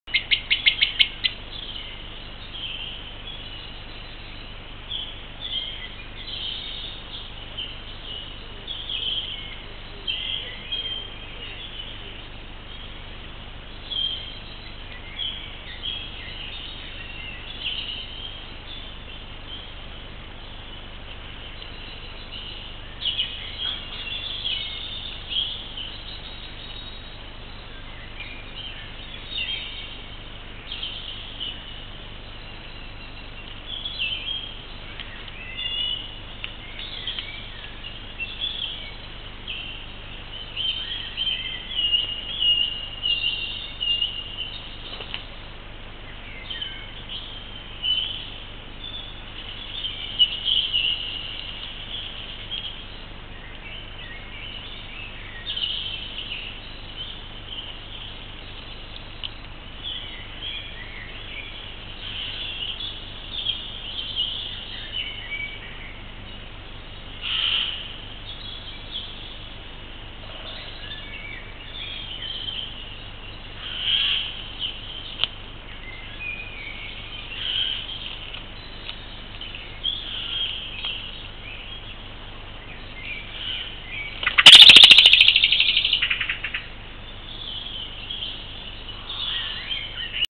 MerelNestmateriaal 2015 03 25 09 47
Terwijl de Oehoes het nog af laten weten verzamelt een Merel al nestmateriaal om 9:47 uur rond de nestholtes. Merel zal wel denken: Als ze hier toch niet willen broeden gebruik ik het wel.